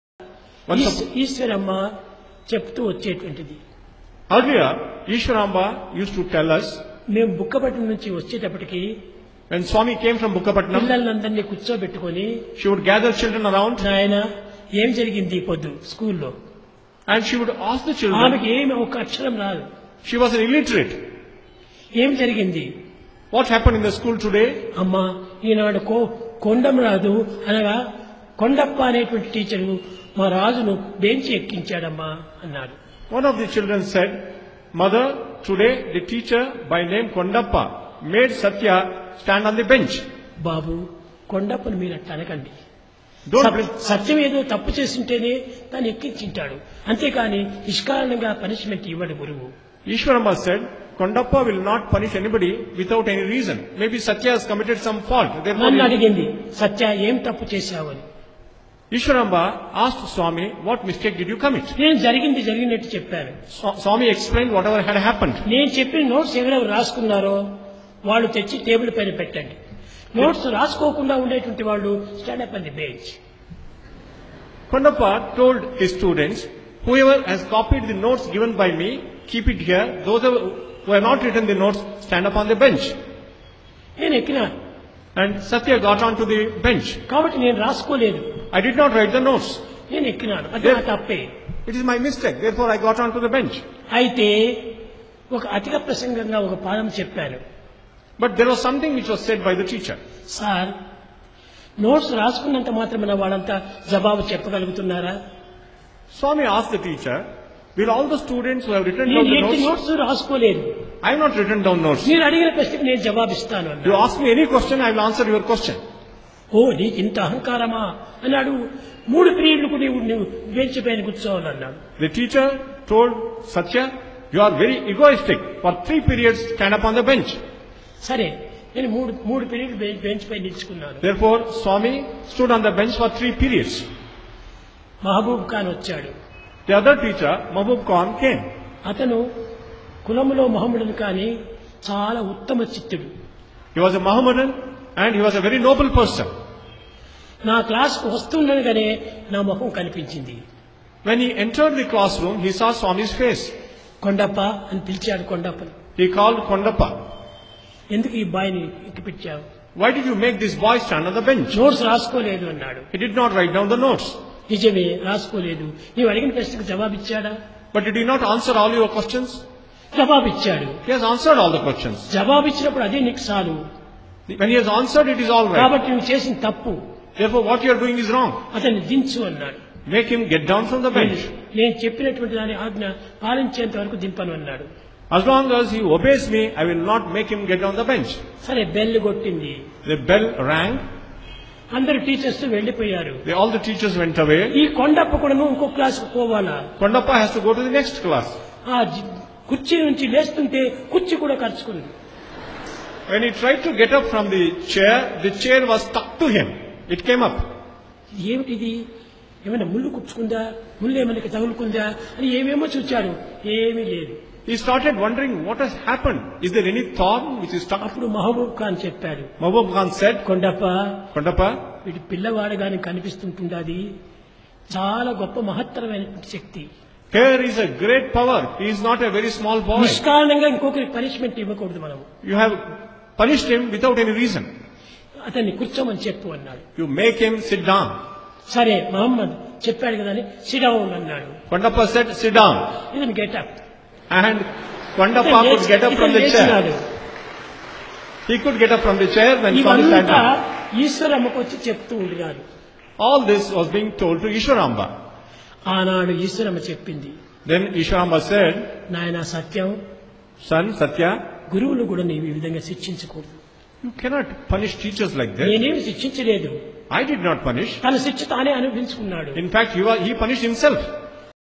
Sathya Sai Relates Childhood Teacher Wrong Punishment Miraculous Experience
Sathya Sai himself relates how as a boy studying in a school, he was wrongly punished by a teacher. And how that teacher got taught a lesson. Swami says that he did not do anything to teach the teacher, the teacher himself brought upon himself the miraculous problem he had to face, which became a lesson for the teacher.